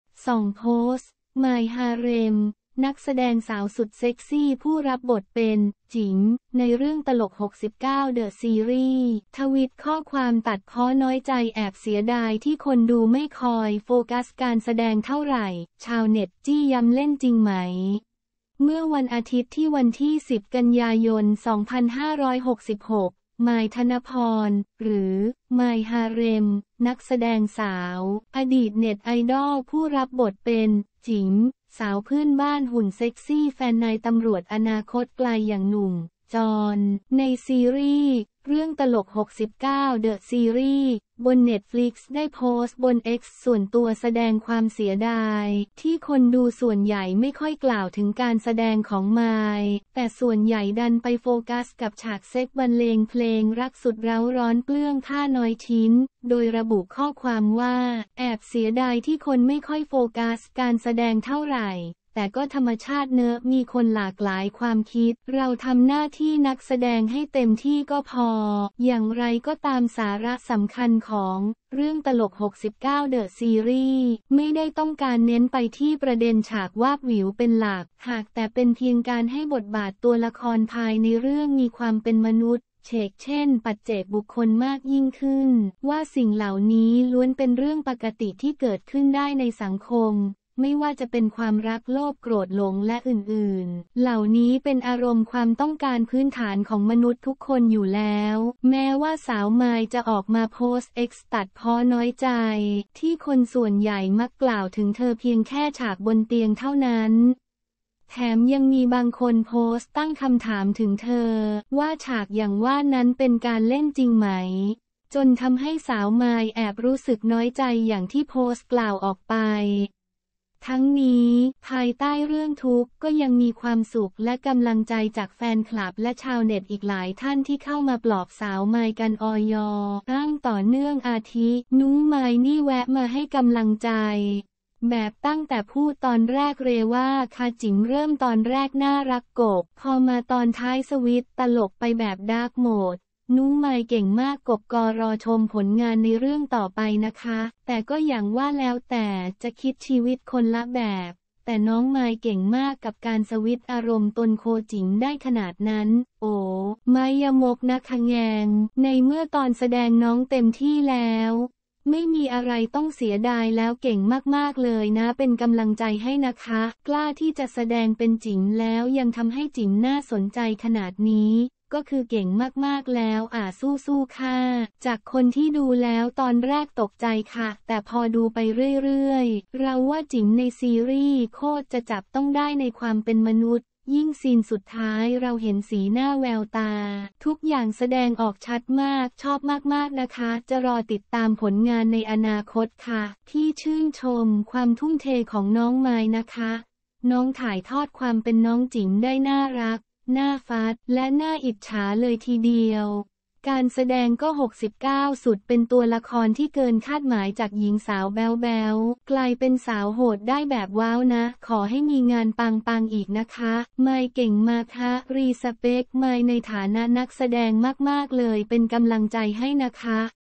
“มายฮาเร็ม” ตัดพ้อ บท “จิ๋ม” เรื่องตลก69 เดอะซีรีส์ คนดูโฟกัสแต่ฉากเปลือย!
ส่องโพสต์ “มาย ฮาเร็ม” นักแสดงสาวสุดเซ็กซี่ผู้รับบทเป็น “จิ๋ม” ในเรื่องตลก69 เดอะซีรีส์ ทวิตข้อความตัดพ้อน้อยใจ แอบเสียดายที่คนดูไม่คอยโฟกัสการแสดงเท่าไหร่ ชาวเน็ตจี้ย้ำเล่นจริงไหม?

เมื่อวันอาทิตย์ที่ 10 กันยายน 2566 “มาย ธนภรณ์” หรือ “มาย ฮาเร็ม” นักแสดงสาว อดีตเน็ตไอดอลผู้รับบทเป็น “จิ๋ม” สาวเพื่อนบ้านหุ่นเซ็กซี่ แฟนนายตำรวจอนาคตไกลอย่างหนุ่ม “จอน” ในซีรีย์ “เรื่องตลก69 เดอะซีรีส์” บนเน็ตฟลิกซ์ ได้โพสต์บนเอ็กซ์ส่วนตัว แสดงความเสียดาย ที่คนดูส่วนใหญ่ไม่ค่อยกล่าวถึงการแสดงของมาย แต่ส่วนใหญ่ดันไปโฟกัสกับฉากเซ็กส์บรรเลงเพลงรักสุดเร้าร้อนเปลื้องผ้าน้อยชิ้น โดยระบุข้อความว่า

“แอบเสียดายที่คนไม่ค่อยโฟกัสการแสดงเท่าไหร่ แต่ก็ธรรมชาติเนอะ มีคนหลากหลายความคิด เราทำหน้าที่นักแสดงให้เต็มที่ก็พอ”
อย่างไรก็ตาม สาระสำคัญของ “เรื่องตลก69 เดอะซีรีส์” ไม่ได้ต้องการเน้นไปเที่ประเด็นฉากวาบหวิวเป็นหลัก หากแต่เป็นเพียงการให้บทบาทตัวละครภายในเรื่องมีความเป็นมนุษย์ เฉกเช่นปัจเจกบุคคลมากยิ่งขึ้น ว่าสิ่งเหล่านี้ล้วนเป็นเรื่องปกติที่เกิดขึ้นได้ในสังคม ไม่ว่าจะเป็นความรัก โลภ โกรธ หลง ฯลฯ เหล่านี้เป็นอารมณ์ความต้องการพื้นฐานของมนุษย์ทุกคนอยู่แล้ว
แม้ว่าสาวมายจะออกมาโพสต์เอ็กซ์ตัดพ้อน้อยใจ ที่คนส่วนใหญ่มักกล่าวถึงเธอเพียงแค่ฉากบนเตียงเท่านั้น แถมยังมีบางคนโพสต์ตั้งคำถามถึงเธอ ว่าฉากอย่างว่านั้นเป็นการเล่นจริงไหม? จนทำให้สาวมายแอบรู้สึกน้อยใจอย่างที่โพสต์กล่าวออกไป ทั้งนี้ ภายใต้เรื่องทุกข์ก็ยังมีความสุขและกำลังใจจากแฟนคลับและชาวเน็ตอีกหลายท่านที่เข้ามาปลอบสาวมายกันอย่างต่อเนื่อง อาทิ

“นุ้งมายนี่แวะมาให้กำลังใจ แบบตั้งแต่พูดตอนแรกเรยว่าคาจิ๋มเริ่มตอนแรกน่ารักกก พอมาตอนท้ายสวิตซ์ตลคไปแบบดาร์กโหมด นุ้งมายเก่งมากกกกกกกก รอชมผลงานในเรื่องต่อไปนะคะ แต่ก็อย่างว่าแล้วแต่จะคิดชีวิตคนละแบบ แต่น้องมายเก่งมากกับการสวิตซ์อารมณ์ตลคจิ๋มได้ขนาดนั้น โอ๋ ๆ นะคะแงง”

“ในเมื่อตอนแสดงน้องเต็มที่แล้ว ไม่มีอะไรต้องเสียดายแล้ว เก่งมากๆ เลยน๊า เป็นกำลังใจให้ นะคะ กล้าที่จะแสดงเป็นจิ๋ม แล้วยังทำให้จิ๋มน่าสนใจขนาดนี้ ก็คือเก่งมากๆแล้วอ่า สู้ๆค่า”
“จากคนที่ดูแล้ว ตอนแรกตกใจค่ะ แต่พอดูไปเรื่อยๆ เราว่าจิ๋มในซีรีส์โคตรจะจับต้องได้ในความเป็นมนุษย์ ยิ่งซีนสุดท้าย เราเห็นสีหน้า แววตา ทุกอย่างแสดงออกชัดมาก ชอบมากๆนะคะ จะรอติดตามผลงานในอนาคตค่ะ”

“พี่ชื่นชมความทุ่มเทของน้องมายนะคะ น้องถ่ายทอดความเป็นน้องจิ๋มได้น่ารัก น่าฟัด และน่าอิจฉาเลยทีเดียว การแสดงก็ 69 สุด เป็นตัวละครที่เกินคาดหมาย จากหญิงสาวแบ๊วๆ กลายเป็นสาวโหดได้แบบว้าวนะ ขอให้มีงานปังๆ อีกนะคะ”

“มายเก่งมากฮะ respect มายในฐานะนักแสดงมากๆ เลย เป็นกำลังใจให้นะคะ”